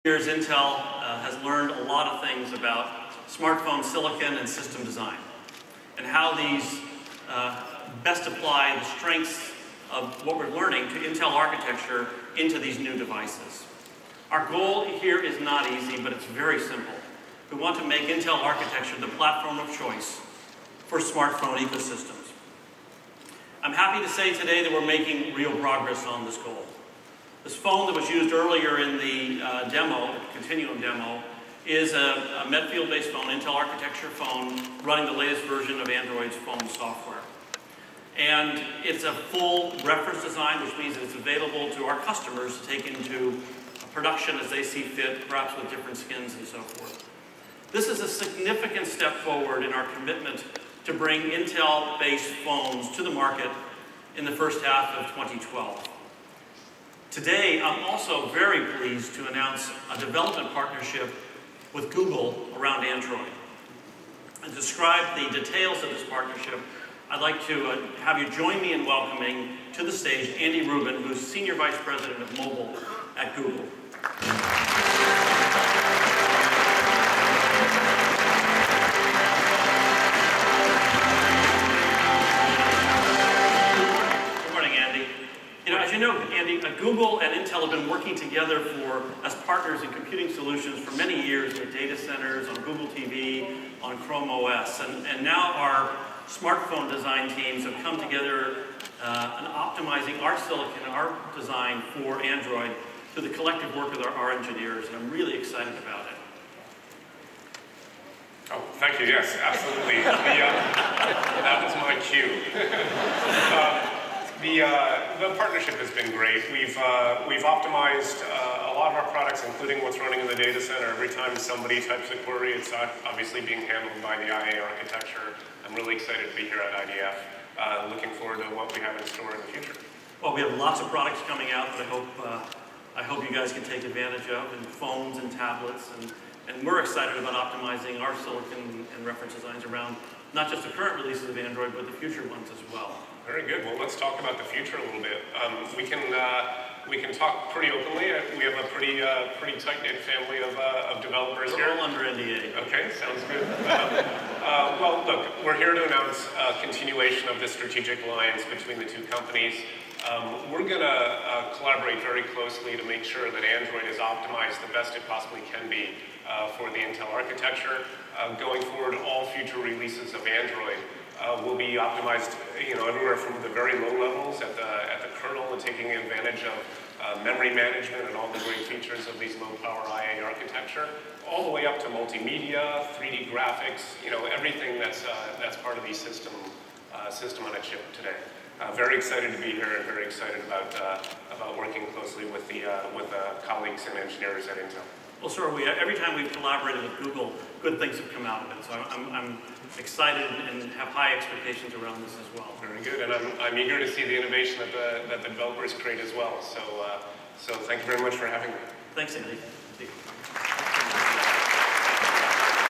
IDF'11 SF: Intel and Google announces Android for Intel Architecture
Andy Rubin, Senior Vice President of Mobile at Google, and Intel President and CEO Paul Otellini announces Android for Intel Architecture